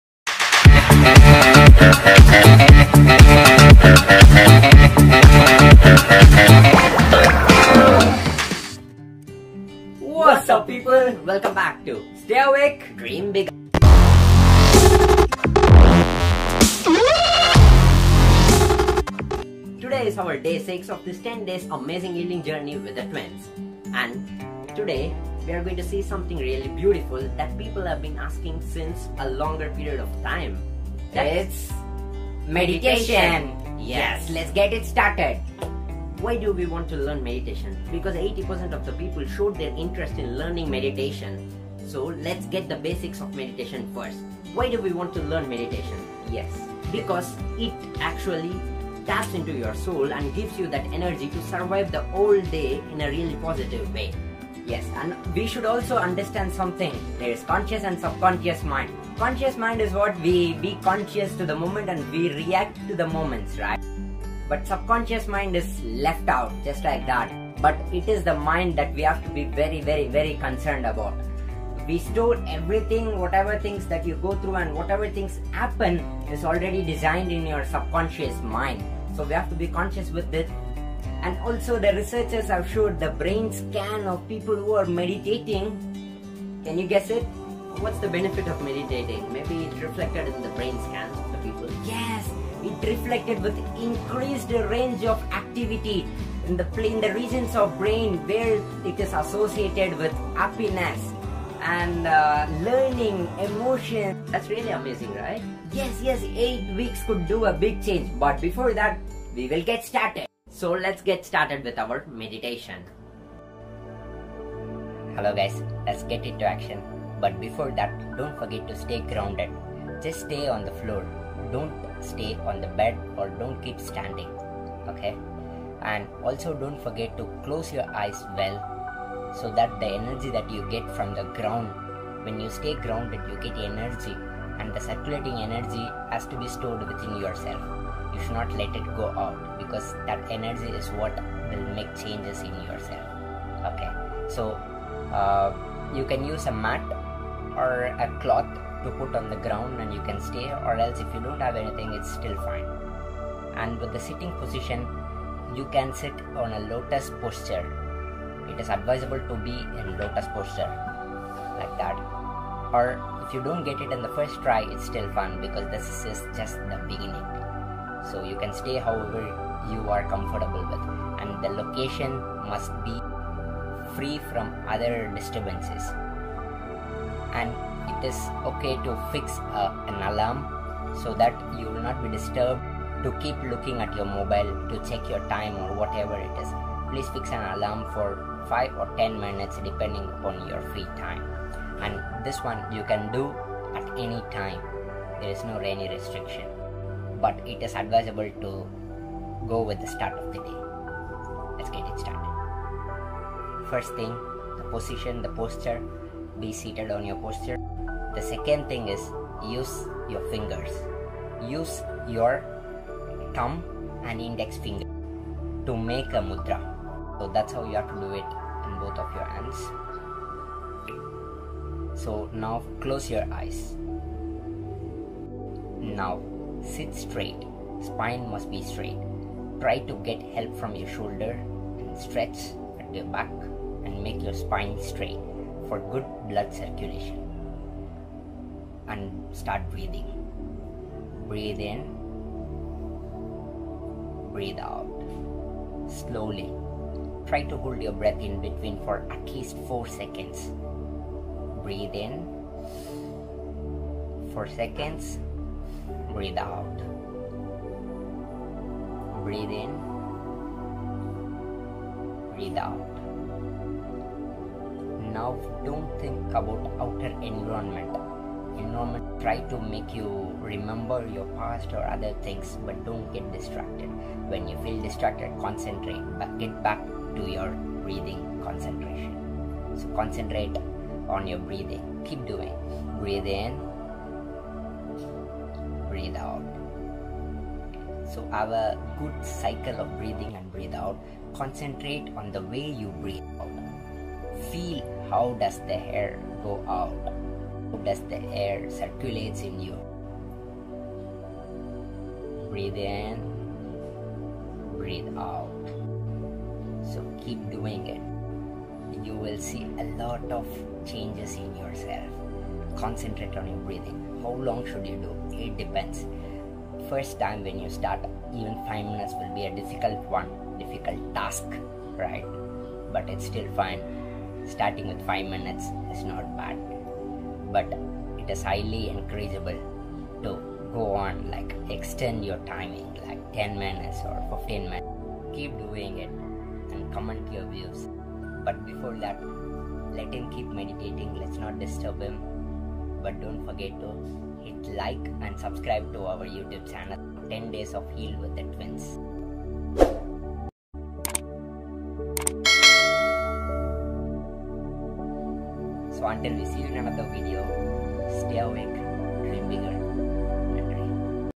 Learn Meditation | Day 06 Heal | Motivation | Stay awake Dream biggger
Meditation starts @1:50

HEAL - 10 DAYS with the twins
DAY 06- In this video we will " learn meditation with breathing exercise"  Don't forget to chant the mandra when you go deeper into meditation .
Where breathing techniques act as an anchor in staying grounded !

Healing is a process and let's get it started!